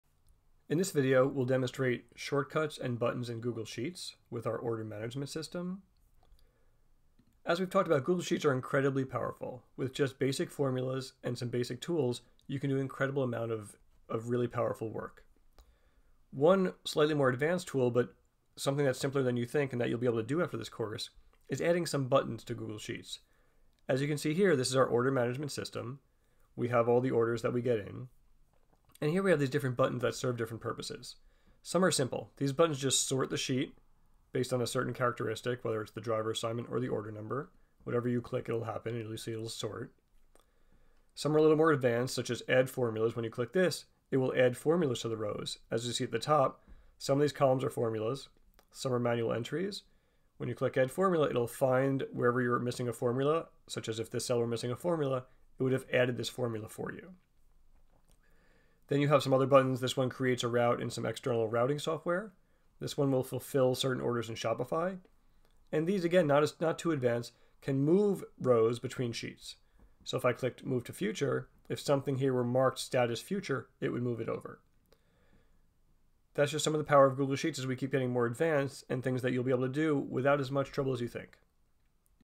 2.6 Shortcuts & Buttons in Google Sheets
Demonstration of sort button and more in Google Sheets